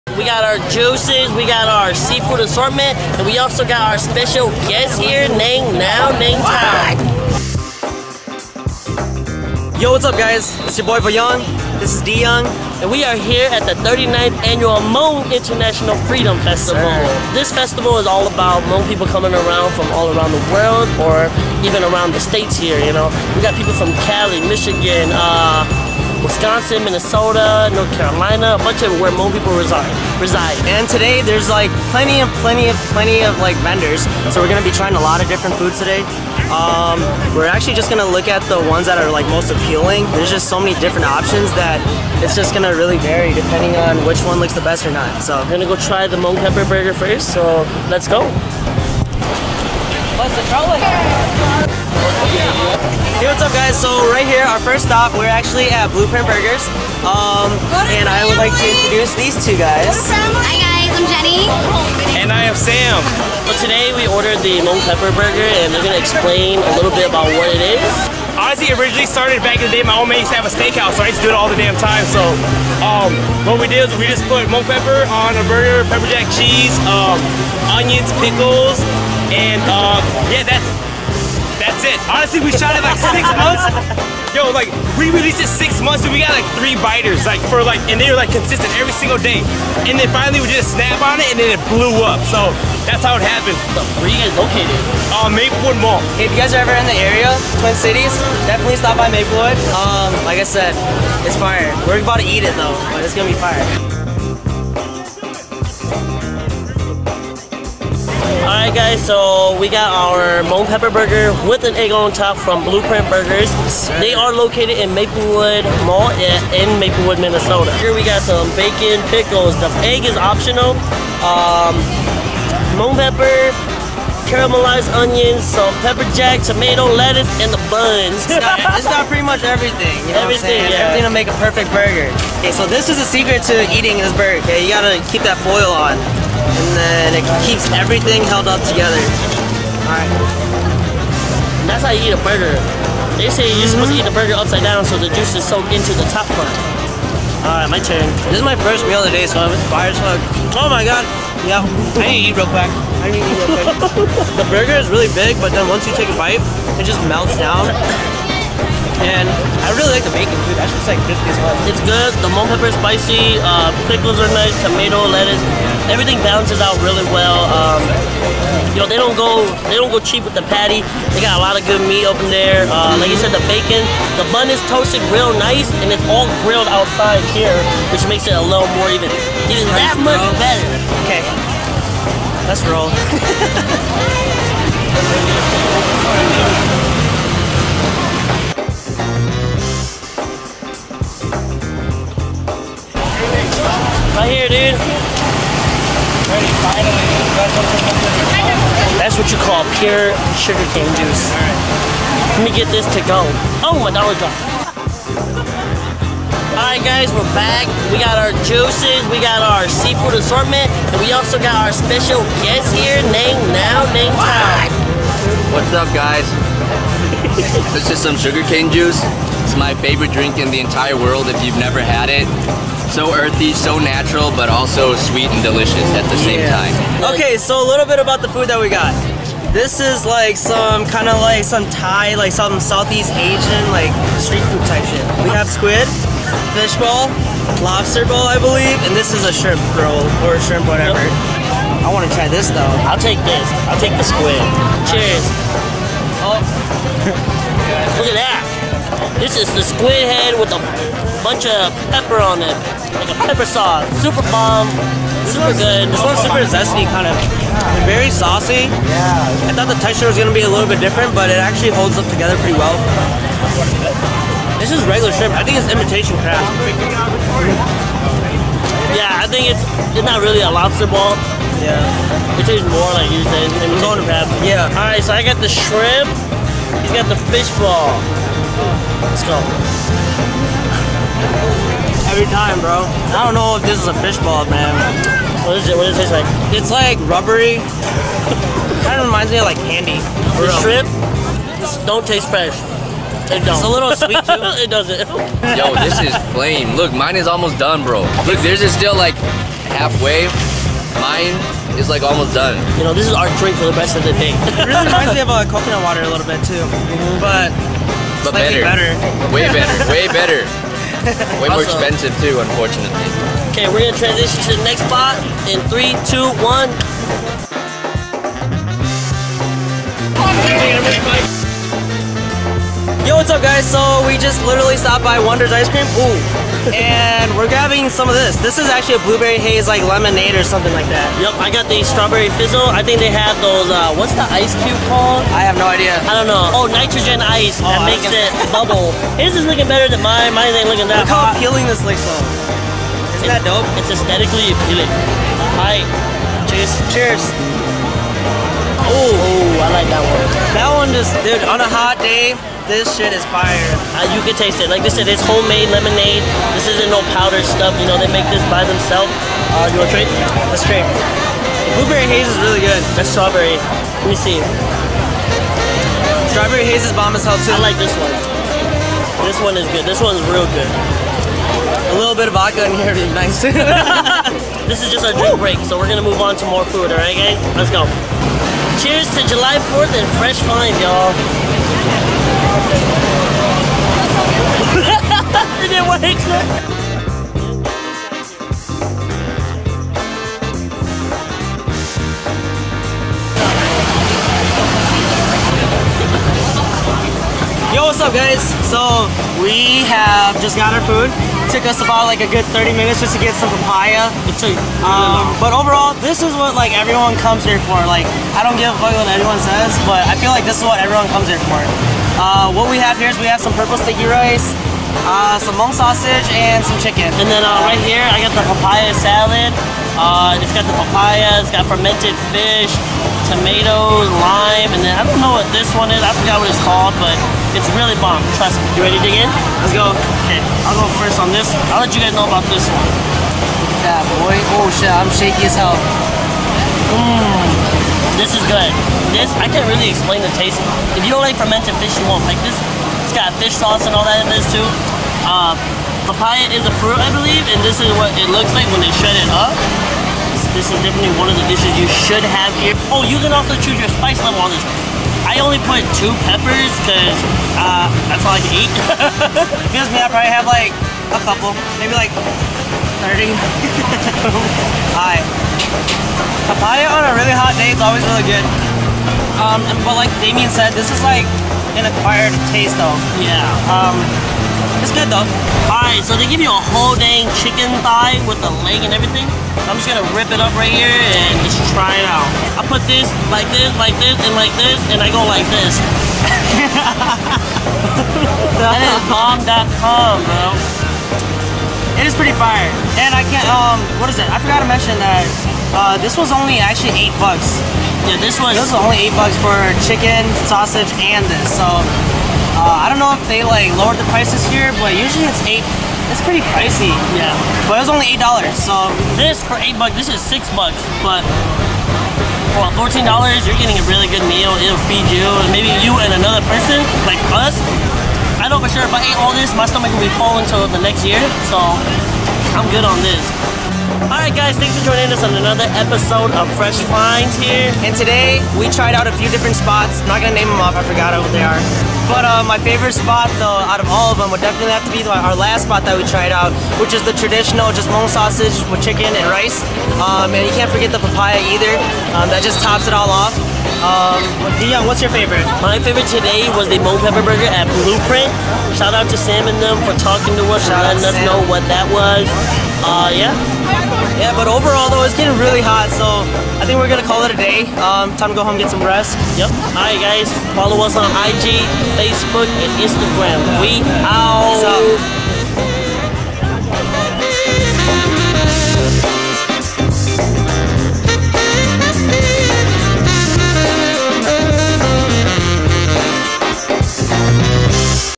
Hmong July 4th Freedom Festival | Fresh Finds Episode 4
Damian Young and VAYOUNG visit the Hmong Freedom Festival on this episode and try everything from a Hmong Pepper Burger, Sugar Cane Juice and many other weird/yummy food! They are also joined by a very special guest in Neng Now!
Instagram.com/eightyfirstave
Twitter.com/eightyfirstave